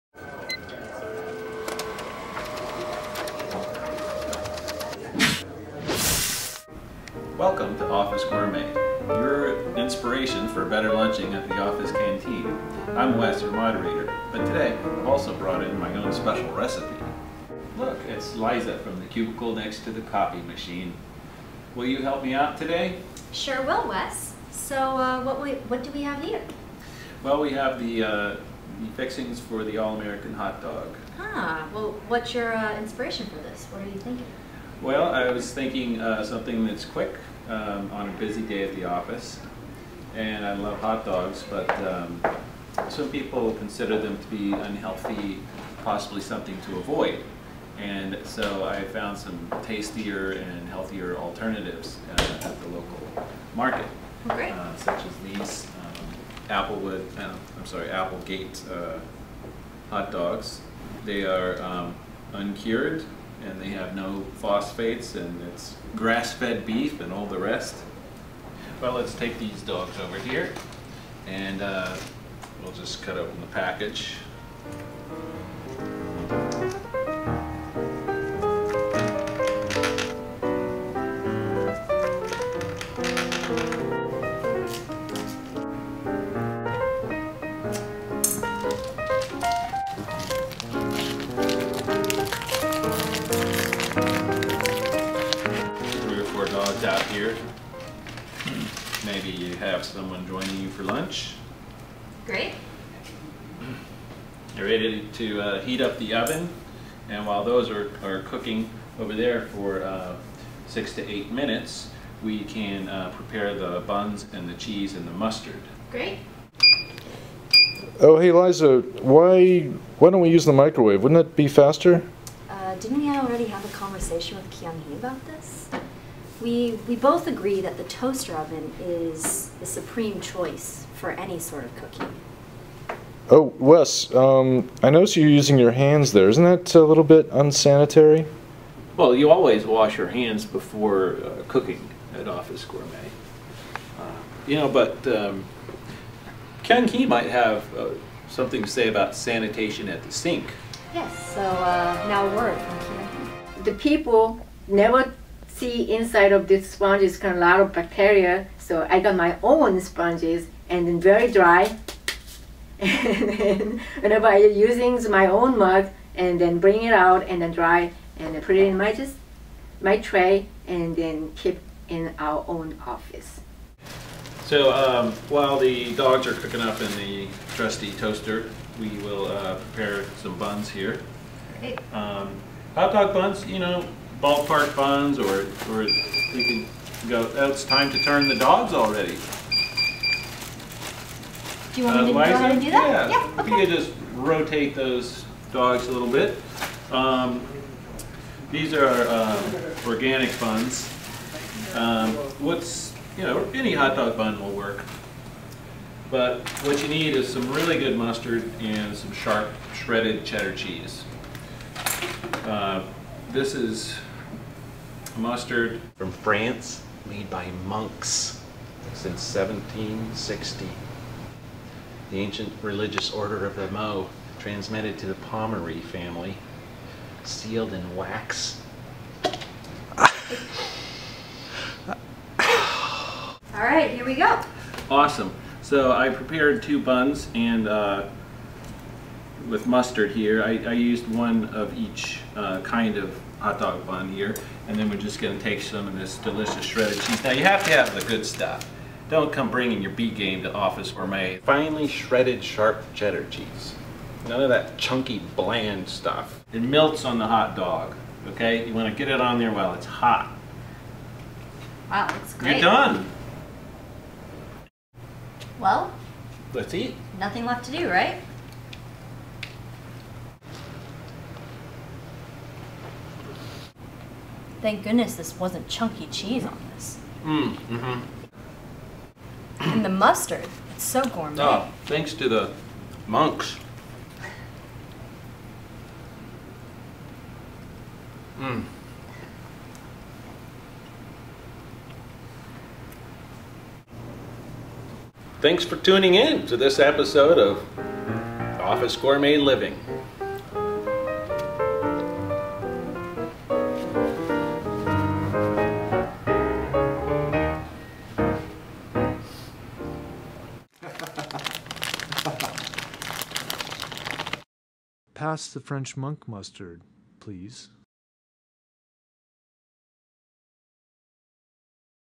Office Gourmet Living, Episode 2: The Office Hot Dog
Office Gourmet Living presents episode two of our web series.  Wes decides to introduce his own version of the office hot dog, with some gourmet surprises.